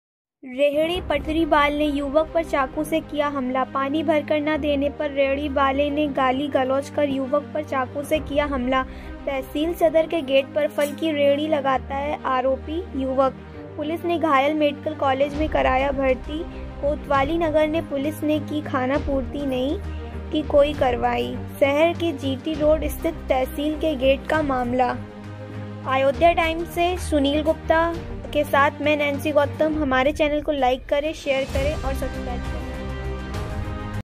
रेहड़ी पटरी बाले ने युवक पर चाकू से किया हमला। DAT News Official | Breaking News | Live
Breaking News LIVE | Top Headlines | Hindi News LIVE | Top News LIVE | Fatafat News | Latest News

Manager - GOLU MOURYA.
Anchor - NAINSI